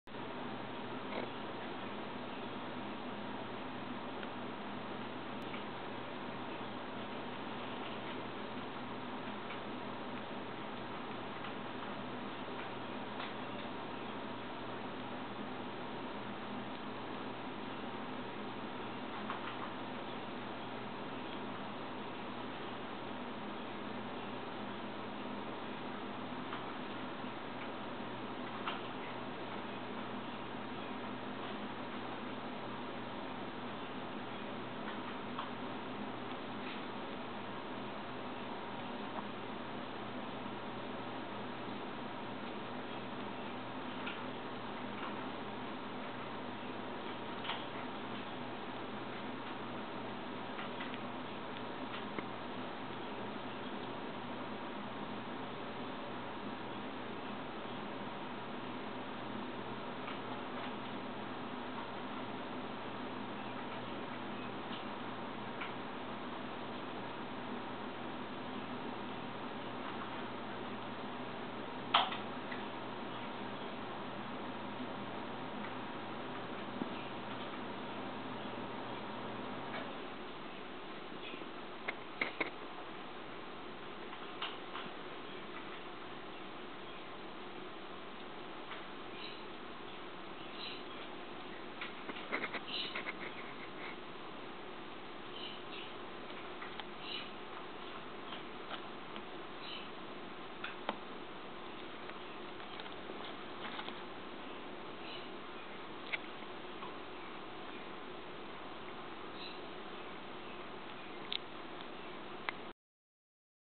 Settling In
I've had my new babies less than 24 hours and they are still really scared.  It will take time to adjust.  Won't come out when I'm around LOL.  Only reason the one was out is because I had cleaned the cage and he was too scared to go back in while I was there HAHA.  Meet Drexel and Butterscotch.